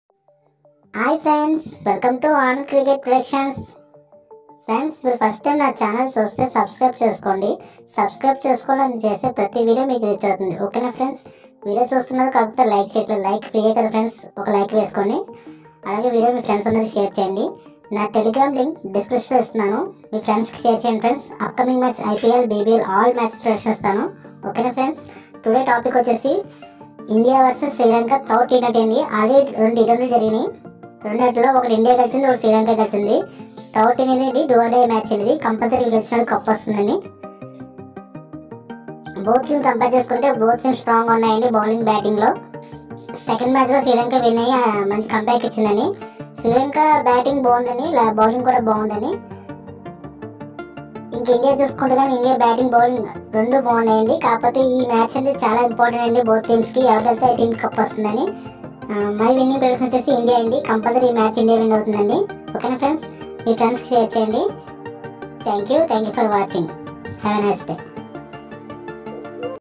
INDIA VS SRILANKA 3rd T20 Match Winner... January 7th 2023@anandcricketpredictions1662
I CAN NOT GUARANTEE THE CORRECT PREDICTION OF THE MATCH AS THE MATCH IS NO FIXED IAM JUST PRIDICTING.IT CAN BE WRIGHT AND IT CAN BE WRONG. BUT IAM RESEARCHING PLAYERS THEIR HISTORY AND TO GET THE RIGHT PREDICTIONS 👍👍